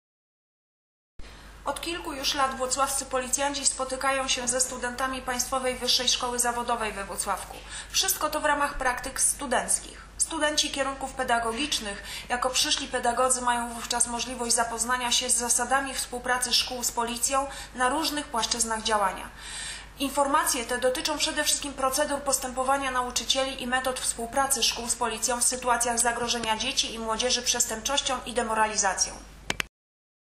Policyjne spotkania ze studentami
Włocławscy policjanci odwiedzili tym razem studentów Państwowej Wyższej Szkoły Zawodowej we Włocławku. A wszystko to w ramach praktyk. Studenci kierunków pedagogicznych, jako przyszli pedagodzy,  mają wówczas możliwość zapoznania się z zasadami współpracy szkół z policją na różnych płaszczyznach działania.
Informacje te dotyczą przede wszystkim procedur postępowania nauczycieli oraz metod współpracy szkół z policją w sytuacjach zagrożenia dzieci i młodzieży przestępczością, a także demoralizacją.
Wyjaśnienie pojęcia osoby nieletniej, czynów karalnych, zagadnień z przepisów prawnych dotyczących zasad postępowania z nieletnimi, to tylko niektóre z poruszanych tematów.
Mamy nadzieję, że przekazane informacje ułatwią współpracę przyszłych nauczycieli i policji.